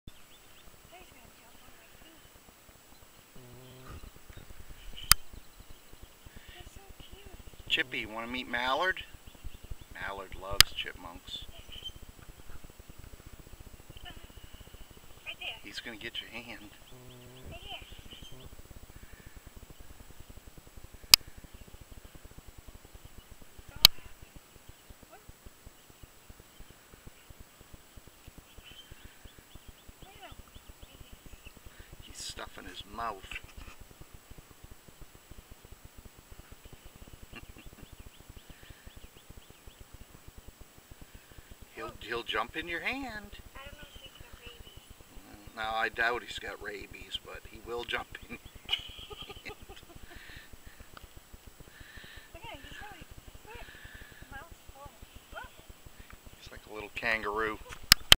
chipmunk &mallard pittsburg nh lake francis
camping trip to lake francis state park & camp ground . mallard chipmunk & red squirrel  fish the lake with a rented boat or just relax enjoy the animals & BIRDS .